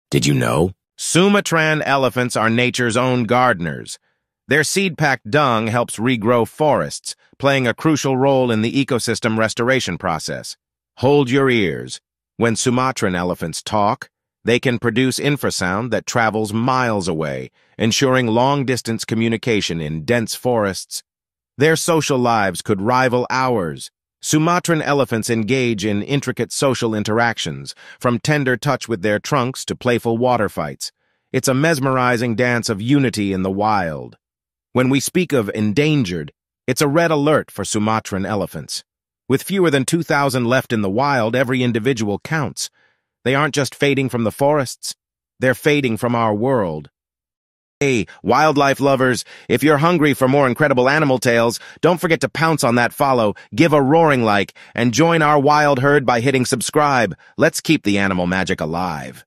Sumatran Elephants: The Silent Extinction NO ONE is Talking About! 😢🐘
Discover the hidden truths about these gentle giants. Time's running out, but can we rewrite their future? Dive in!